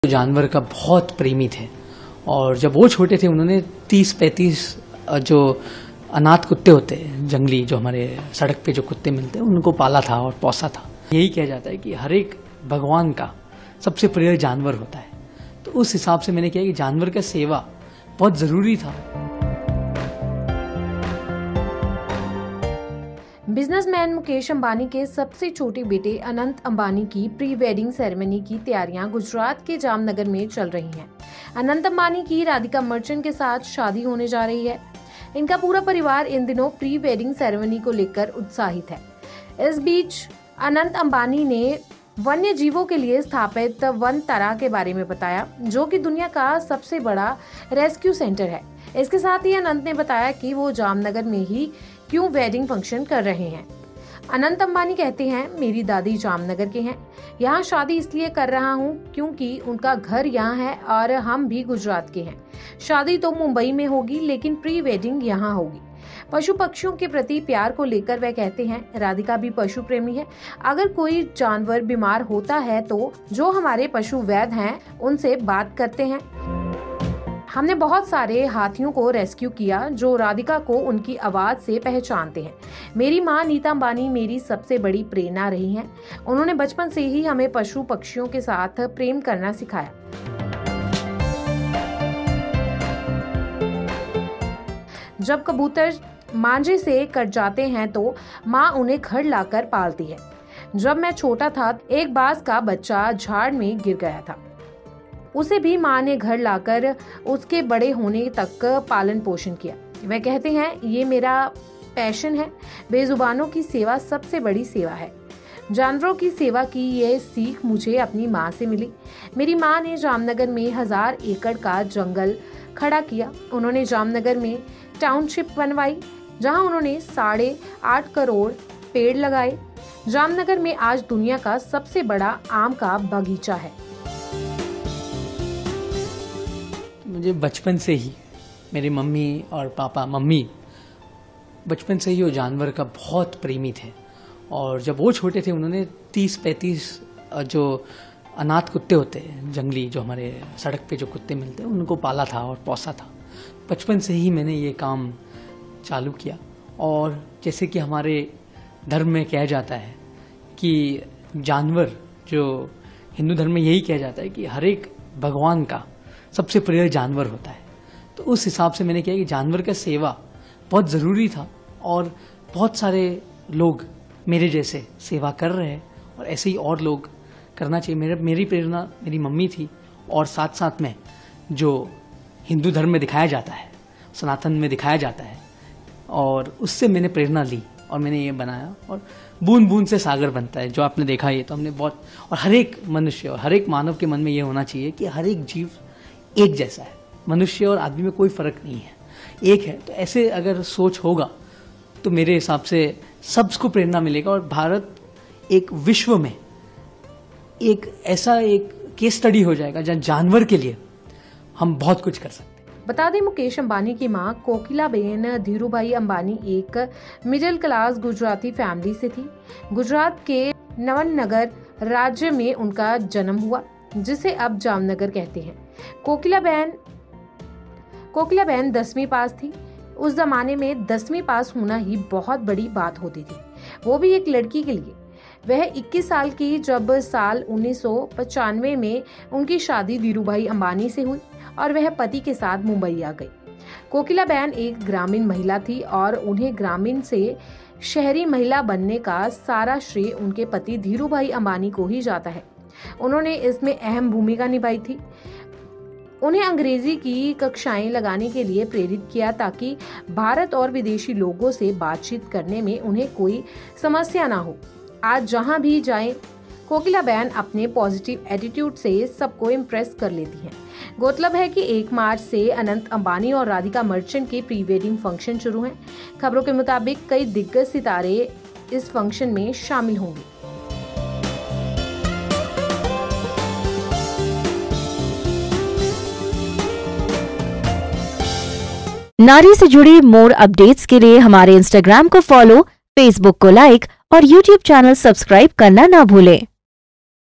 Radhika Merchant की आवाज पहचानते हैं कई हाथी, मुंबई छोड़ क्यों कर रहे जामनगर में शादी
‘इनकी खास सेवा करना चाहते हैं Anant Ambani’ मुंबई नही जामनगर में ही शादी करना चाहते थे Mukesh के छोटे लाड़ले, दादी Kokilaben से जुड़ी है वजह.